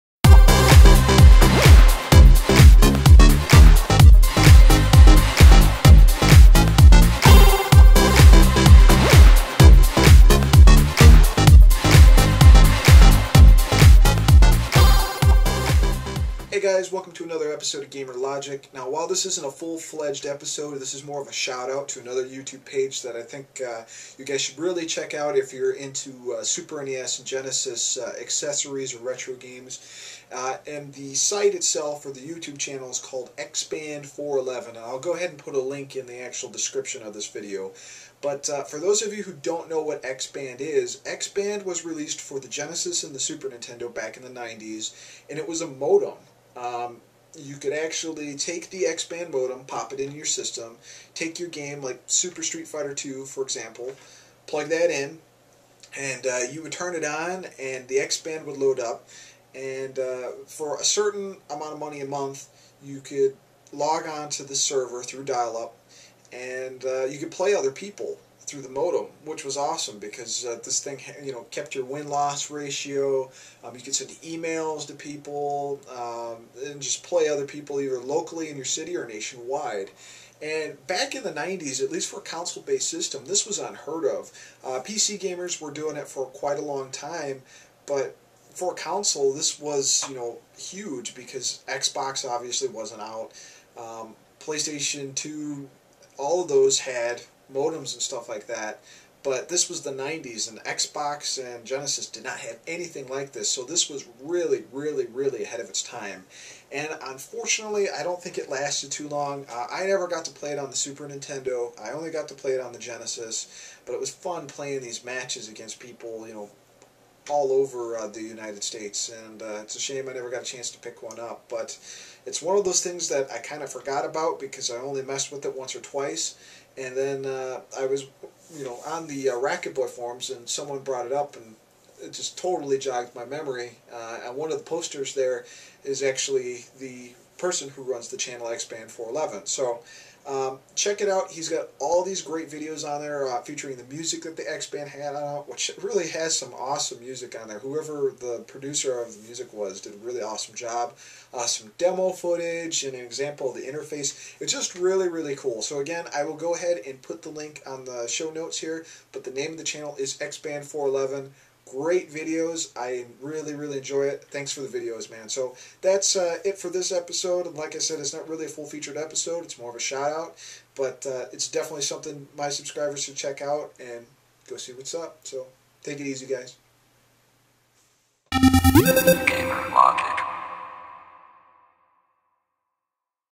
Gamer Logic: The Xband Modem
A recent forum discussion really jogged my memory about this SNES/Genesis add on called the Xband. Xband was a modem that allowed you to do some pretty cool things on your system!

Check out the Xband411Youtube channel for info on it! Xband411's page has pretty much everything you need to know!